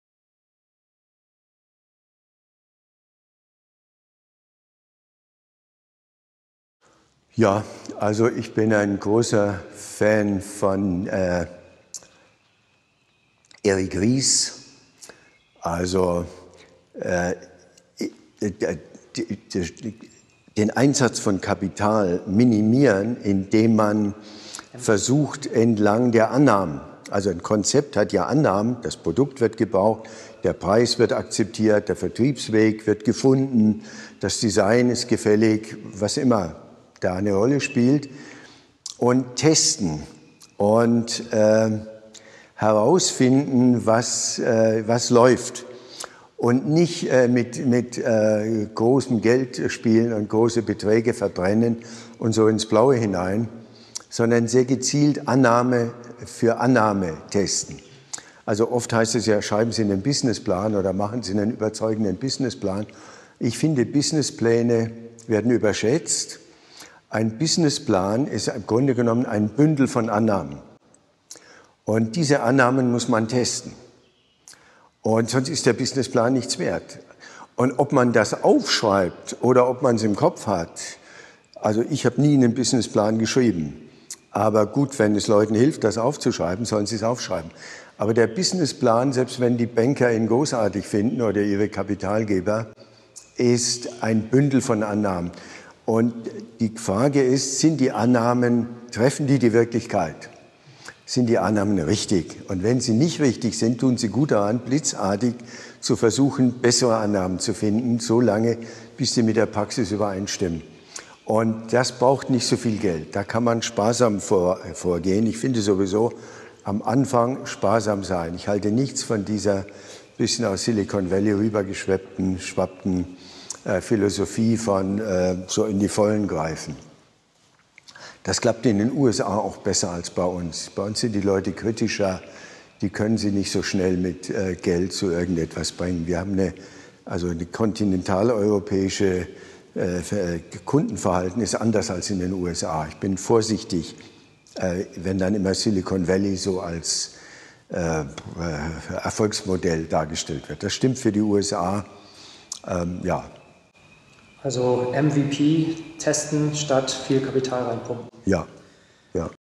Interview mit Prof. Günter Faltin - Was ist Ihr “Must-Read” für Unternehmer?
Was ist Ihr “Must-Read” für Unternehmer?

Interview mit Prof. Günter Faltin (Autor von Kopf schlägt Kapital, Tee-Kampagne, Entrepreneurship-Summit, etc.). Ende Januar 2021 durfte CC Gründer Tim Chimoy Prof. Günter Faltin in seinem Haus in Chiang Mai mit den Fragen aus der Community interviewen. Sein umfangreiches Gründer-Wissen und seine Erfahrungen gibt er gerne an Interessierte weiter. Im Interview beantwortet er Fragen zum Unternehmer Mindset und gibt hilfreiche Tipps für Entrepreneure.